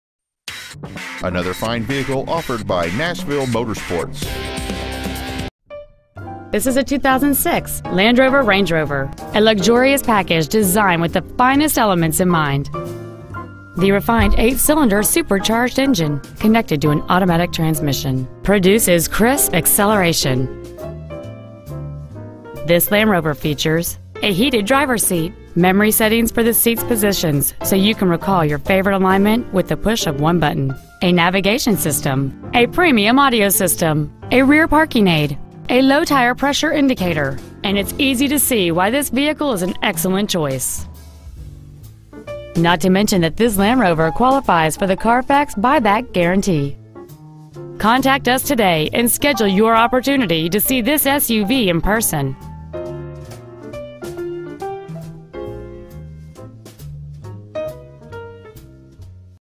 Pre-Owned 2006 Land Rover Range Rover Nashville TN
We are proud to present this 2006 Land Rover Range Rover.

 Year: 2006
 Make: Land Rover
 Model: Range Rover
 Engine: 4.2L SMPI 32-valve supercharged aluminum alloy V8 engine
 Trans.: Automatic
 Exterior: White
 Miles: 72,950
 Interior: Tan

 2222 8th Ave. South

 Used 2006 Land Rover Range Rover Nashville TN